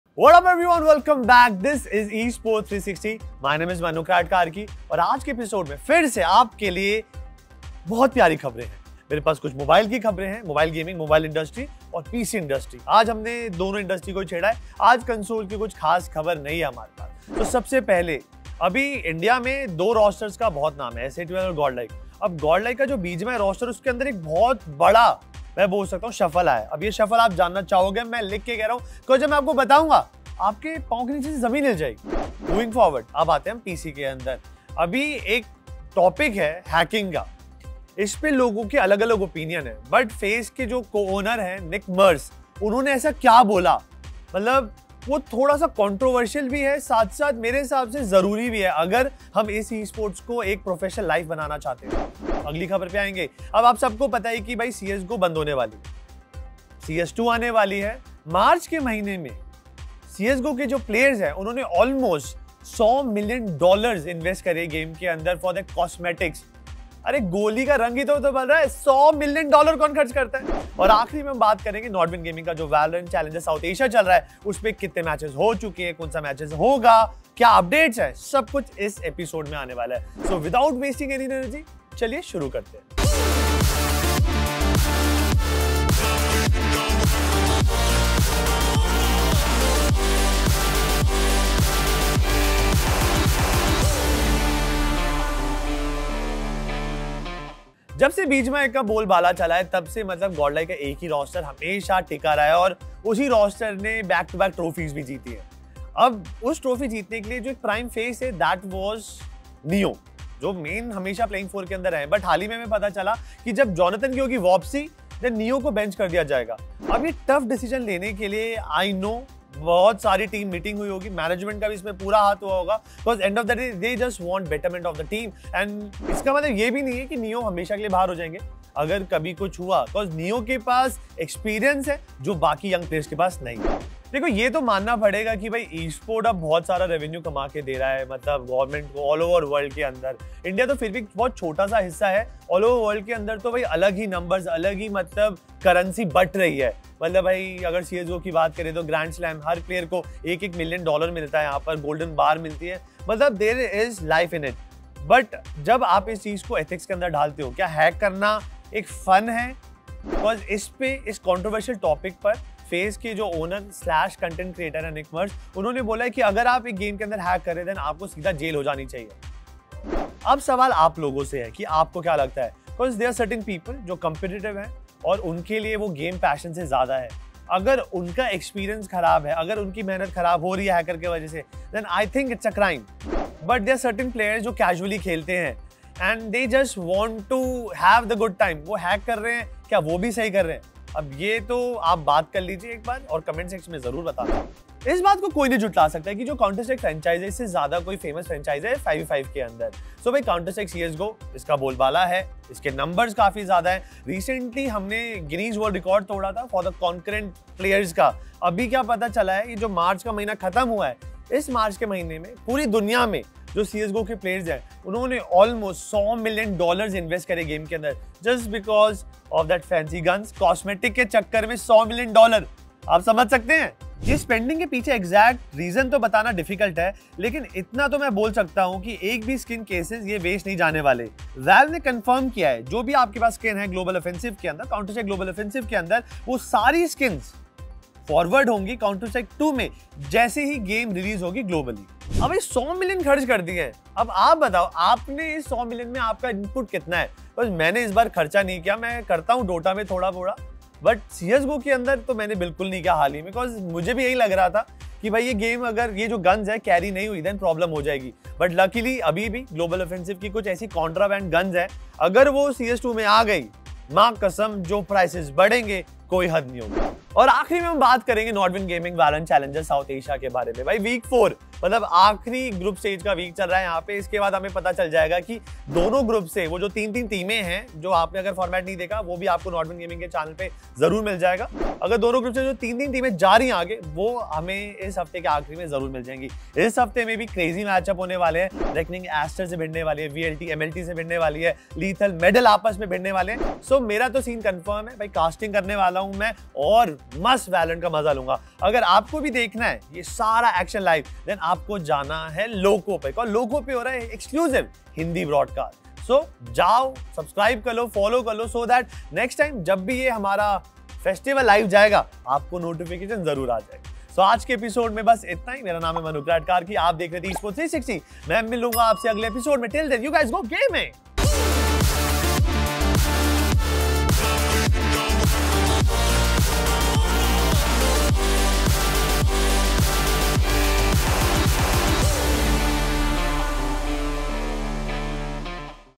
CSGO $100M in Cosmetic Spending, GodLike Neyo Benched? Nickmercs talks about Cheaters! | Esports 360
Welcome to Gaming News Esports 360, your ultimate source for all the latest gaming news! In this edition, we bring you some exciting updates from the world of esports. First up, we discuss the amazing news that CSGO has reached a milestone of $100M in cosmetic spendings. Next, we dive into the rumors surrounding the benched position of GodLike Neyo from the team. We also bring you the latest statement made by Nickmercs against cheaters in esports. And last but not least, we provide you with the updates from the Nodwin Valorant Challengers South Asia.